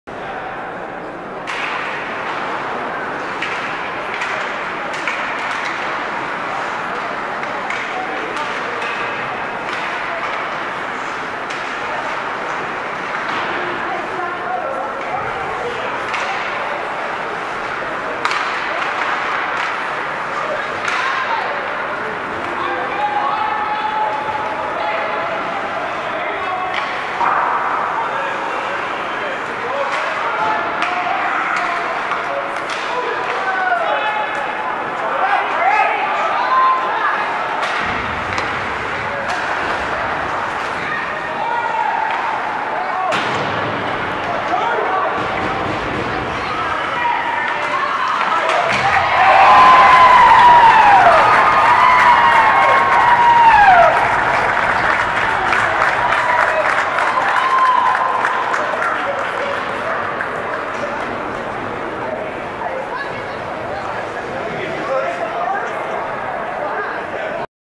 Marist HS Hockey Joe Prkut #38 Goal- Ryan Butt # 93 Assist
vs Mt. carmel 12-13-15